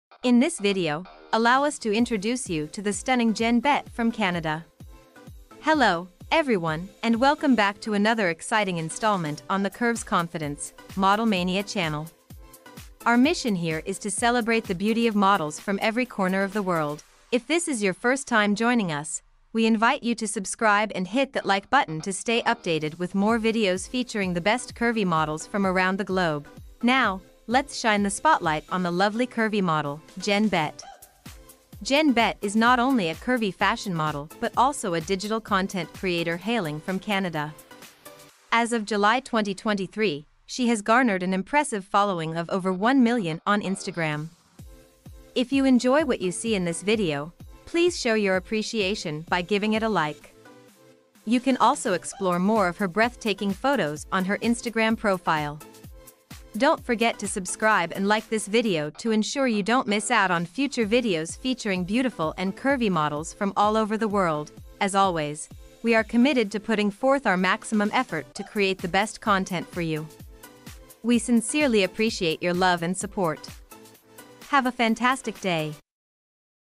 Jen Brett From Canada
* All Models Channel *

- In This Video We Date Jen Brett From Canada

- Curvy Plus Size Model - Beauty Influencer - Model Icon - Fashionista And Many More